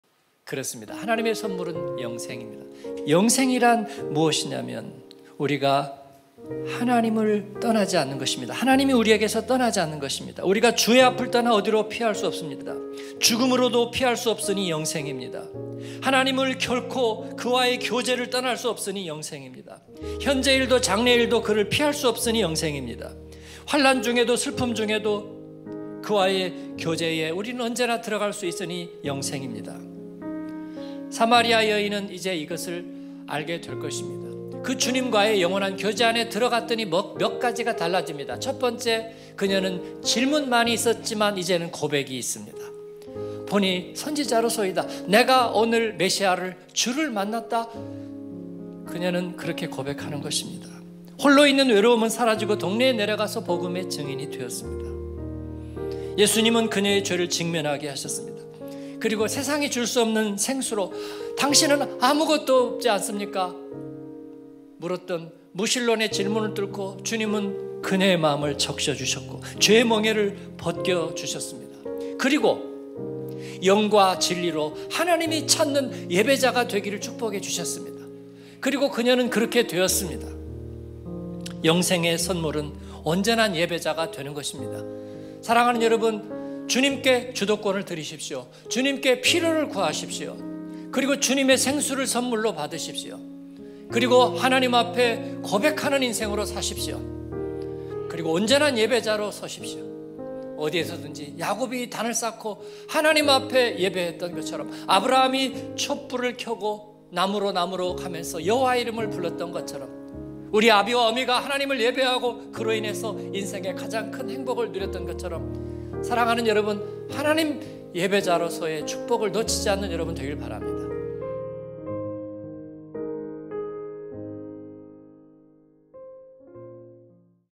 이찬규 목사 5분 말씀 | 2022.11.20 | 주님을 향한 네 가지 질문 | 요한복음 4장 10절
주님을 향한 네 가지 질문 (요한복음 4장 10절)